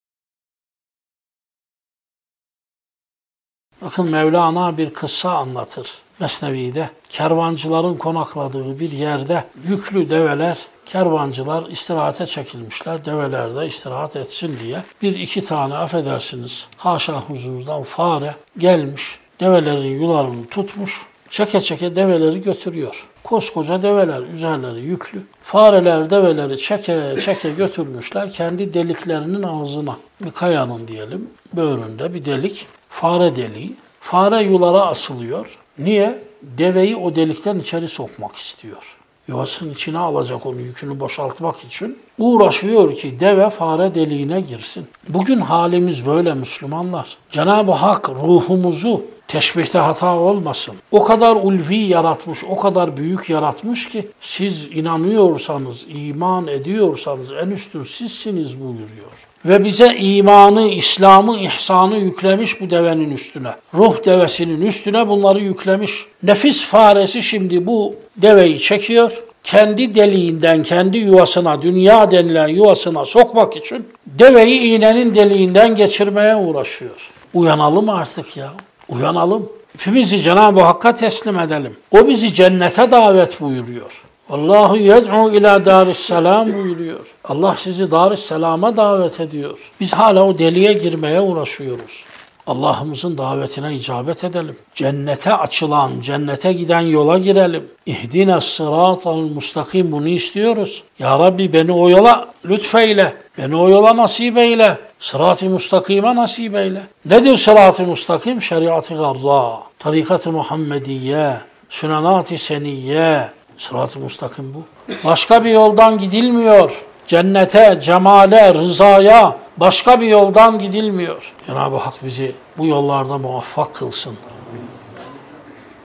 Mesnevi | Ruh Devesi Nefis Faresi | Yakub Haşimi Hocaefendi (ksa)
Devenin Yularını Çeken Fare

Sohbetin tamamına aşağıdaki linkten ulaşabilirsiniz
Tarikate İntisab Niçin Lazımdır - Yakub Haşimi Hocaefendi (ksa)